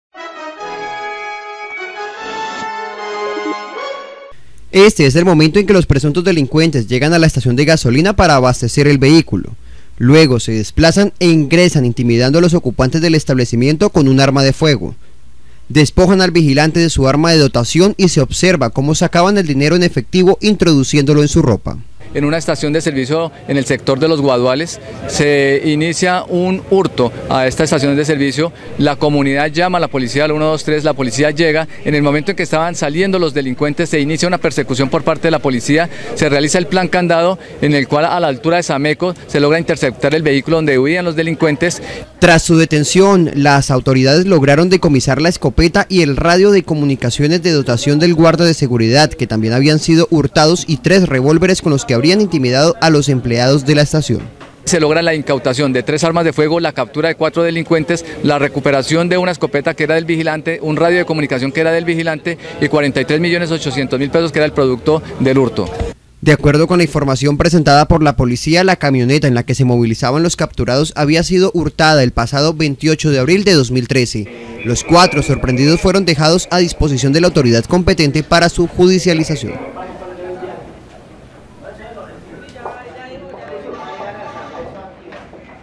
En video quedó registrado el robo a estación de gasolina en Cali
Este es el momento en que los presuntos delincuentes llegan a la estación de gasolina para abastecer el vehículo, luego se desplazan e ingresan intimidando a los ocupantes del establecimiento con un arma de fuego. Despojan al vigilante de su arma de dotación y se observa como sacaban el dinero en efectivo introduciéndolo en su ropa.

Tras su detención, las autoridades lograron decomisar la escopeta y el radio de comunicaciones de dotación del guarda de seguridad que también habían sido hurtados y tres revólveres con los que habrían intimidado a los empleados de la estación.

De acuerdo con la información presentada por la Policía  la camioneta en la que se movilizaban los capturados había sido hurtada el pasado 28 de abril de 2013. Los 4 sorprendidos fueron dejados a disposición de la autoridad competente para su judicialización.